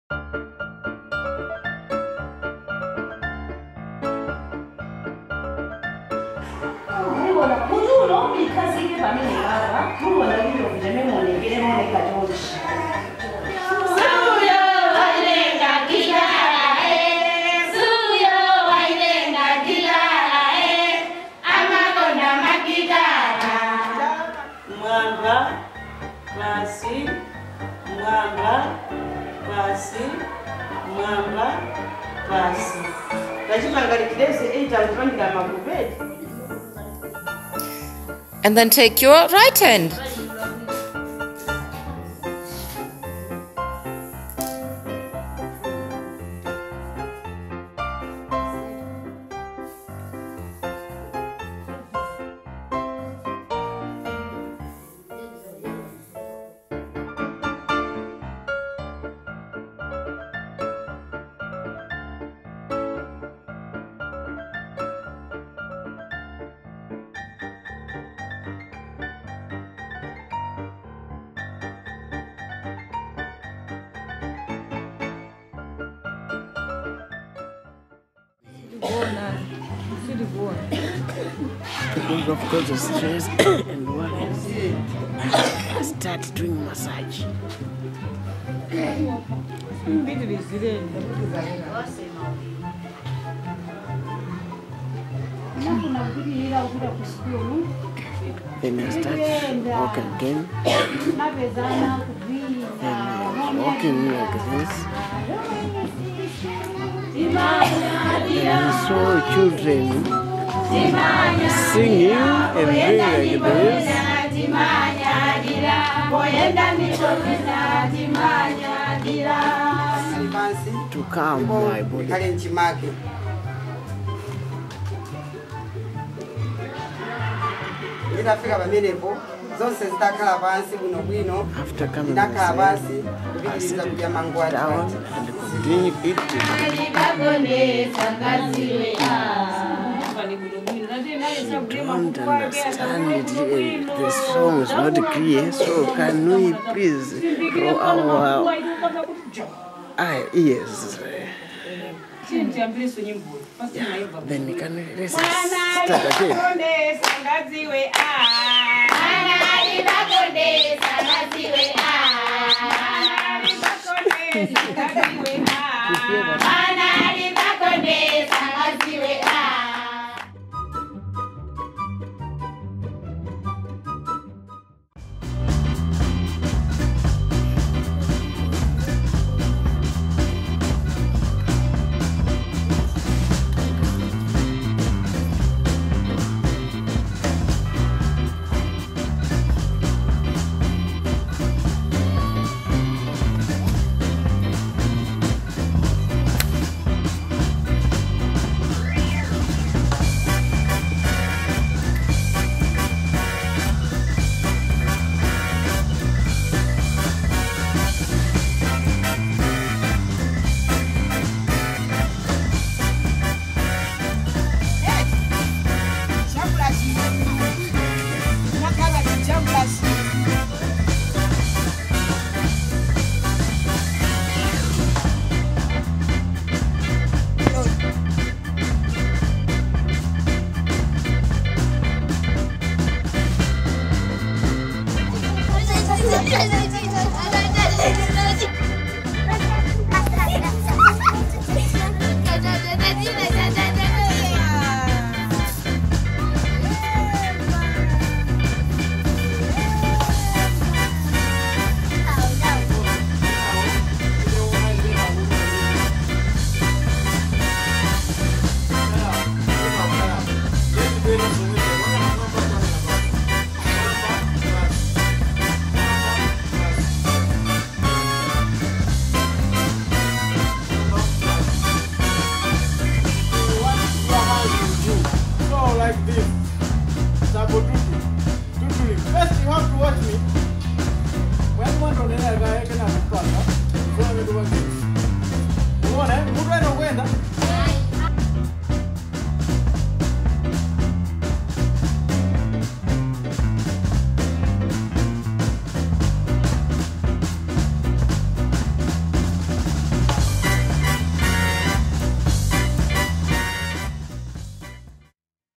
Brain Gym in Malawi
Teaching Experience of the Brain Gym programme with Nursary School Teachers and Educators in Malawi